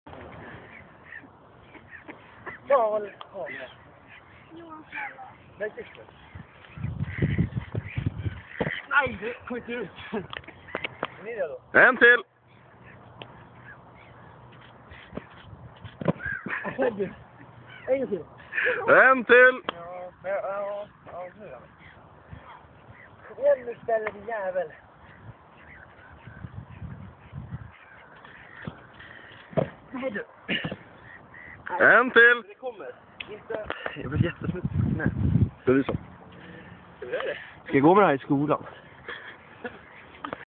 jag gör en såndär fejsplant
lite spontan voltning på rasten